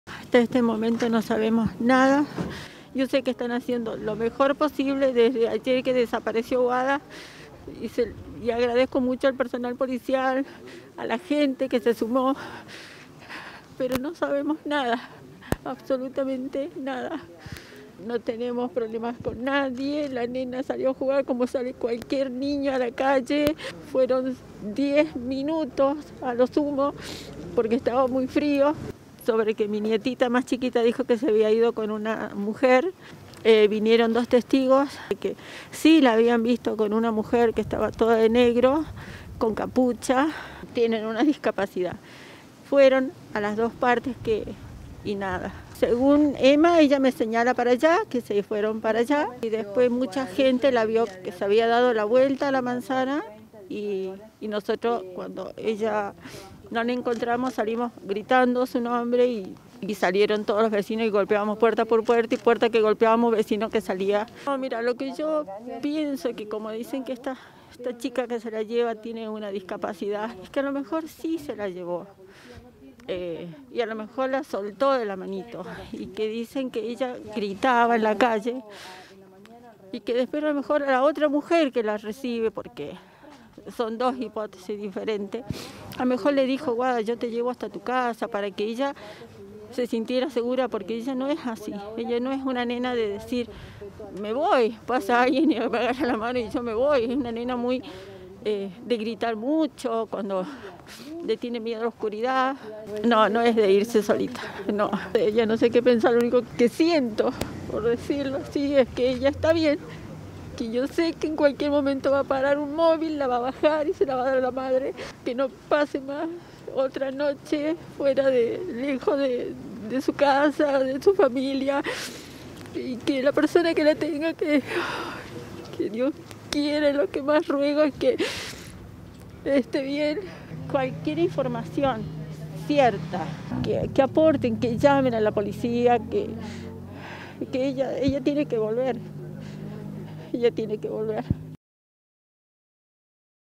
INTENSA BÚSQUEDA DE UNA NENA DE 5 AÑOS
Esta tarde dialogamos con la abuela de la pequeña Guadalupe que es intensamente buscada por la policía desde que desapareció del barrio 544 Viviendas Sur. Hay testimonios de vecinos que coinciden en haberla visto con una mujer que vestía de negro.